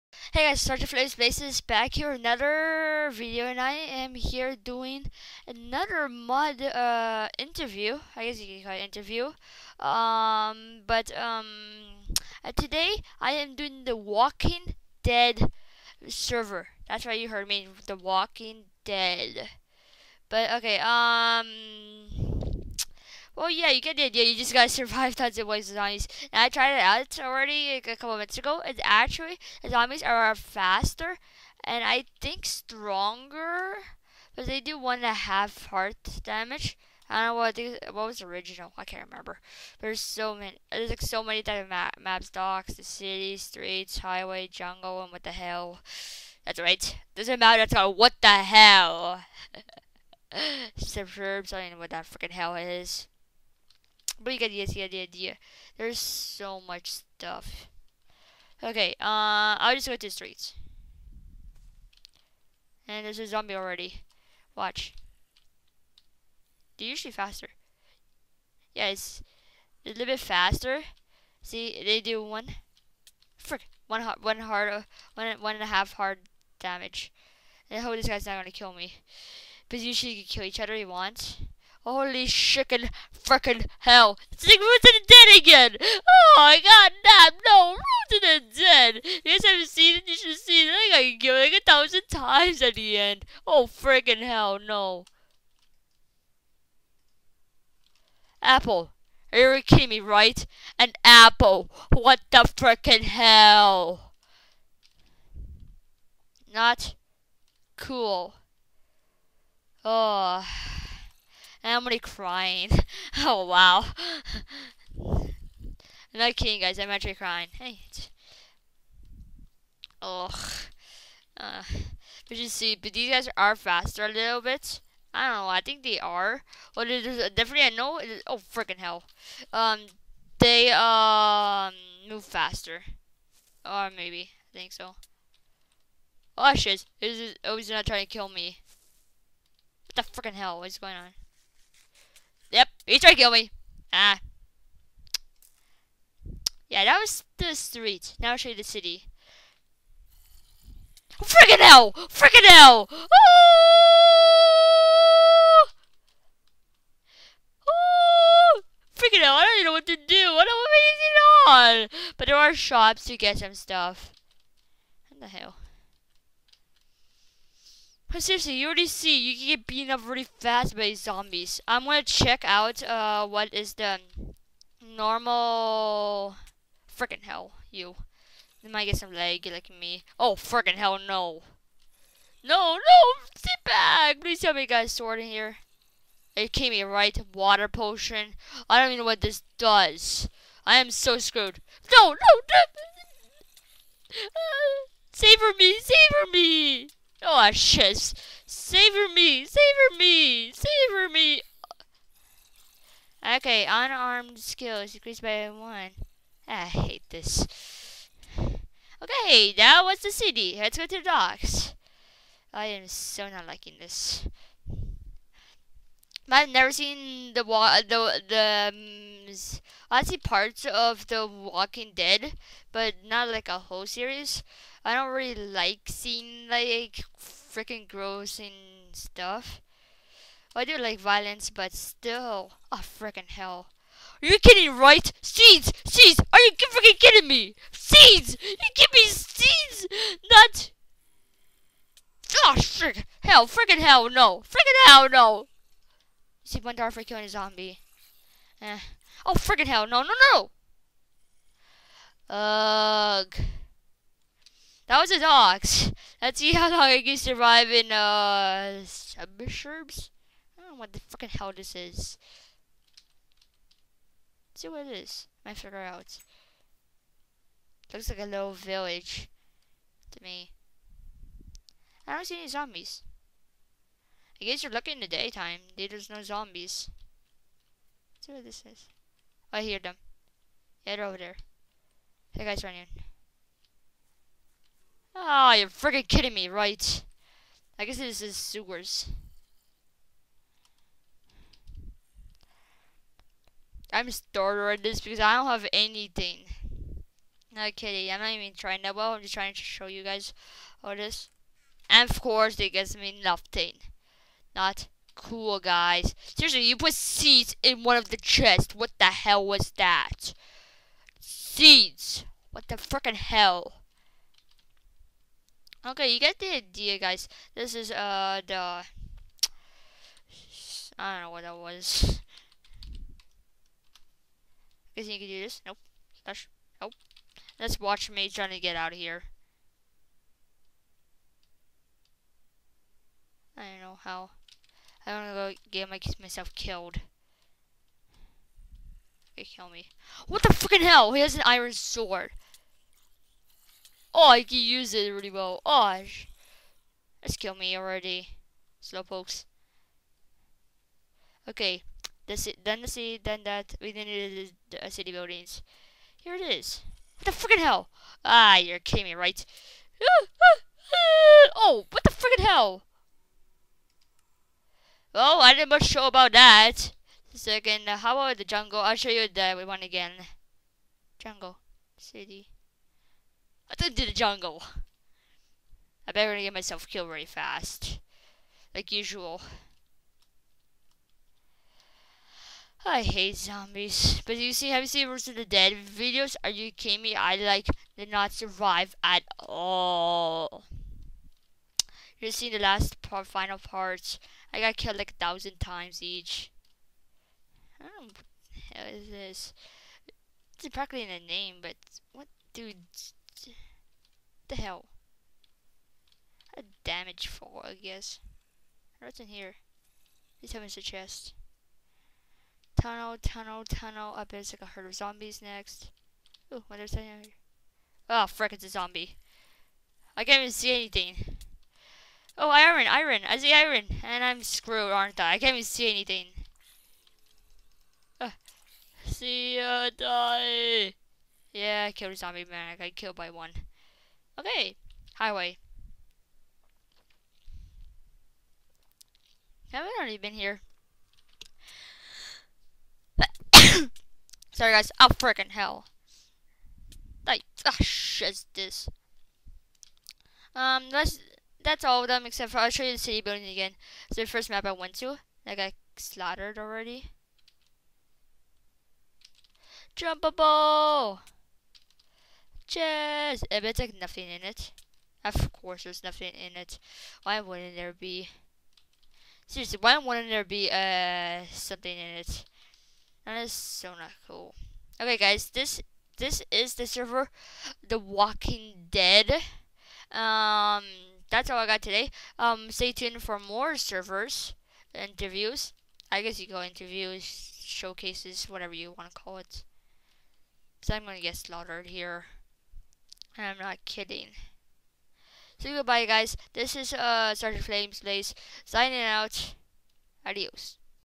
Server Interview (The Walking Dead)
This the Waling dead server where zombies are a bit stronger.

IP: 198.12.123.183